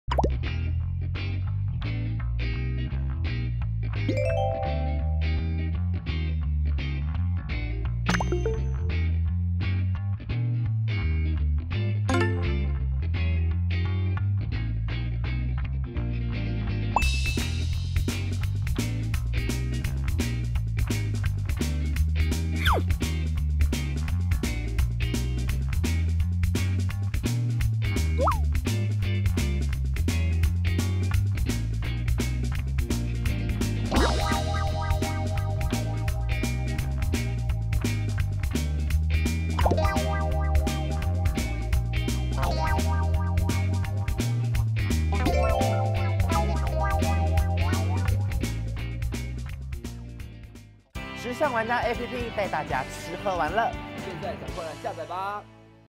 必勝客「台味第一讚鹽酥雞比薩」開賣！爽吃整圈OREO餅皮，還有魷魚圈和甜不辣
2天1夜go👉週三晚間7點